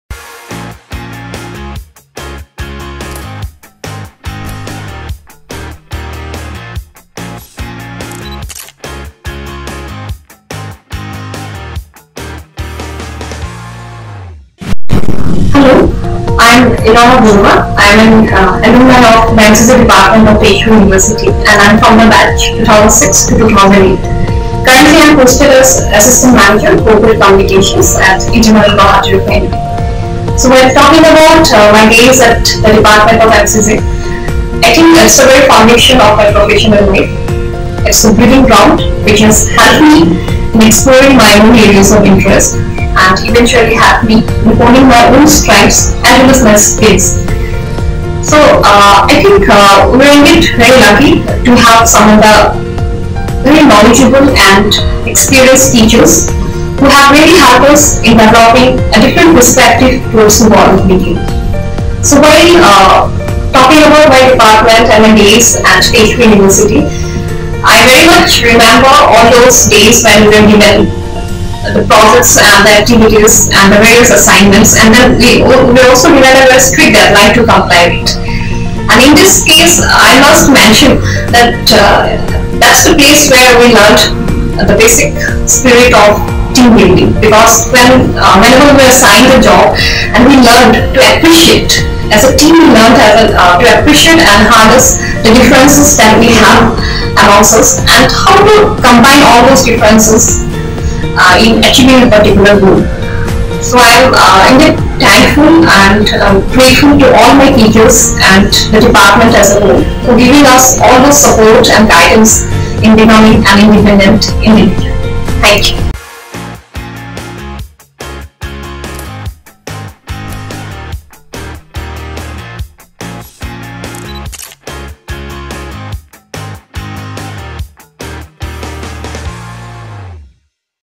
TU MCJ ALUMNI SPEAK - Elora Baruah (2006 - 2008 Batch)
Assistant Manager, Corporate Communication, IOCL Guwahati Refinery